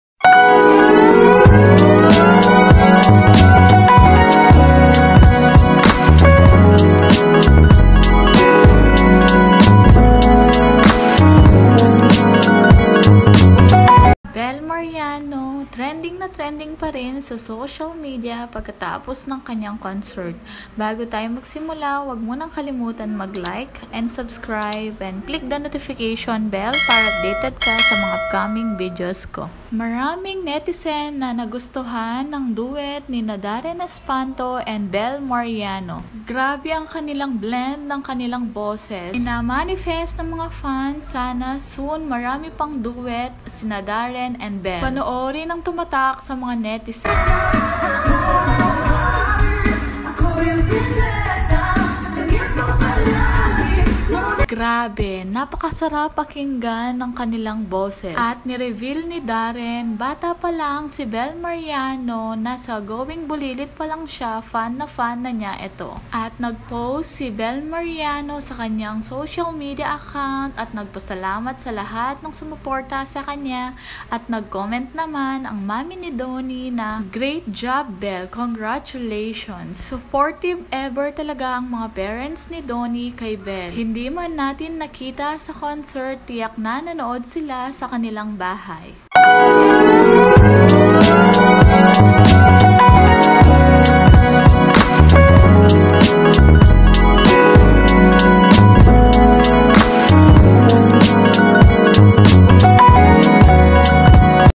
MASAYANG INUWI NI BELLE M. ANG PICTURE NI DONNY P.
Darren espanto and belle mariano nagustuhan ang kanilang duet noong nakaraang sabado minamanifest ng mga netizen na marami pang duet na kanta ang dalawa  m